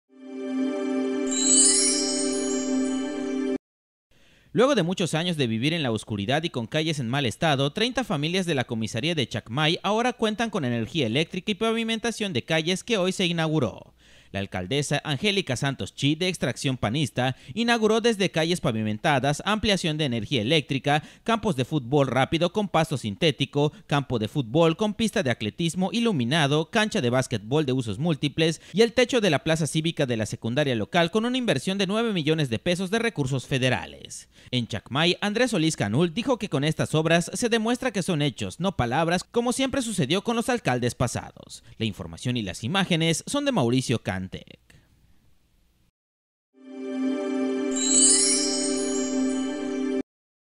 Inauguran obras en Dzoncauich
La alcaldesa de Dzoncauich, Angelica Santos Chi, inauguró diversas obras en la localidad, con una inversión de 9 millones de pesos de recursos federales